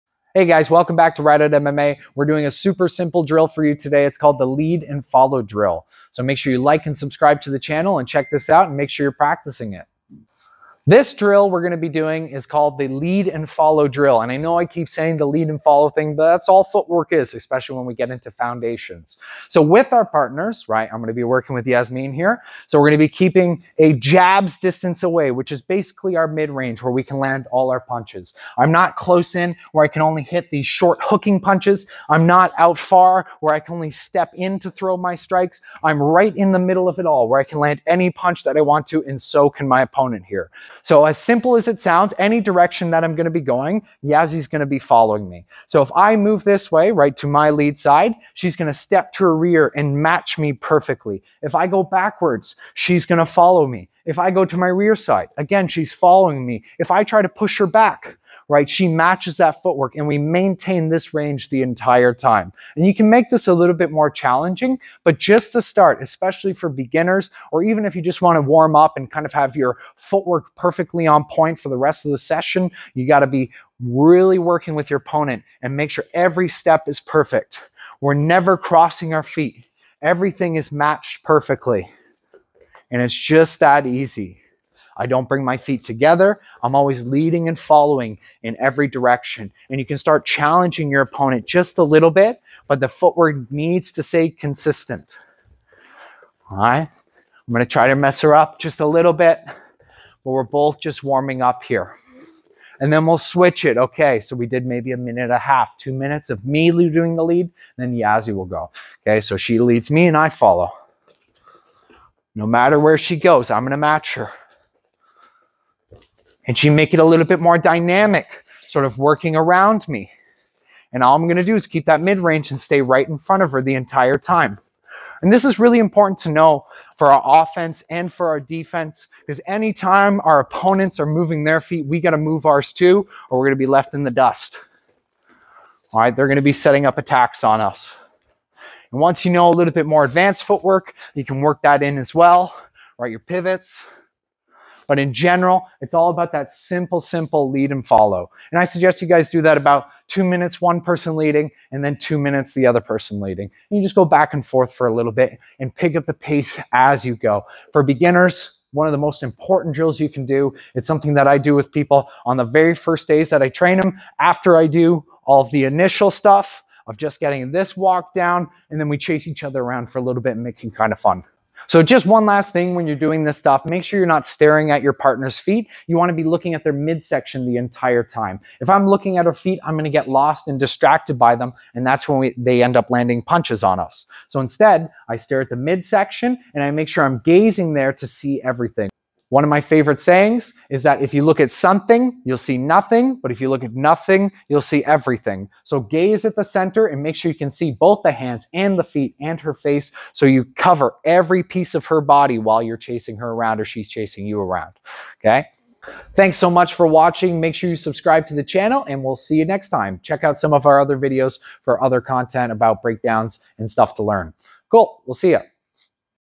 Boxing Footwork Drills: Perfect Footwork for Beginners
Improve your footwork in minutes with the super simple drill, the lead and follow drill. Grab a partner and spend a few minutes working on your movement to establish your fundamentals.